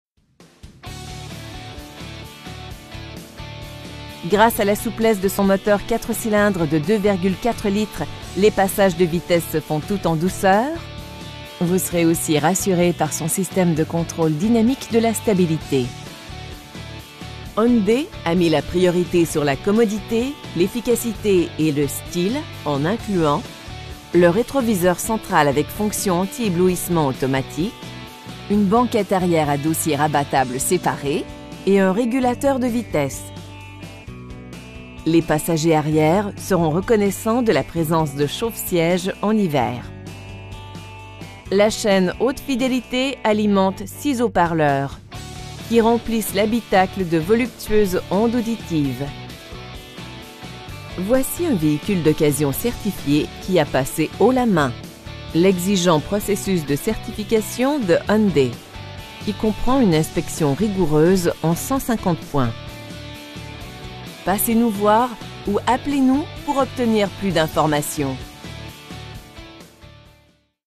2014 Hyundai Tucson GLS AWD TOIT OUVRANT BLUETOOTH MAGS
Brossard Hyundai
8750, Boul Taschereau

Nous avons tout un spécial sur le Hyundai Tucson 2014. Affichant moins de 60 000 km au compteur, ce VUS 4 portes met la priorité sur le confort, la sécurité et son côté pratique. Grâce à la souplesse de son moteur 4 cylindres de 2,4 litres, les passages de vitesses se font tout en douceur, vous serez aussi rassuré par son système du contrôle dynamique de la stabilité. Hyundai a mis la priorité sur la commodité, l'efficacité et le style en incluant: un compte-tours, un ordinateur de bord, le rétroviseur central avec fonction antiéblouissement automatique, des sièges baquets à l'avant, une banquette arrière à dossiers rabattables séparés, phares à contrôle automatique, et un régulateur de vitesse. Les passagers arrière seront reconnaissants de la présence de chauffe-siège en hiver. Le toit ouvrant à commande électrique permet de se rapprocher de la nature et d'humer l'air frais en un tour de main. La chaîne haute fidélité alimente 6 haut-parleurs, qui remplissent l'habitacle de voluptueuses ondes auditives. Hyundai accorde la priorité à la sécurité et la protection en incluant: coussins gonflables à l'avant, rideaux gonflables pour protéger la tête, système antipatinage, système d'assistance au freinage, appuis-tête avant anti-coup de fouet, un avertisseur d'urgence, et freins à disque aux 4 roues avec antiblocage. Le sophistiqué système de traction intégrale procure une tenue de route rassurante par toutes conditions routières. Voici un véhicule d'occasion certifié qui a passé haut la main l'exigent processus de certification de Hyundai, qui comprend une inspection rigoureuse en 150 points! Nous le savons, vos attentes sont élevées. Voilà notre défi : les satisfaire et les surpasser à tout prix! Passez nous voir ou appelez-nous pour obtenir plus d'information.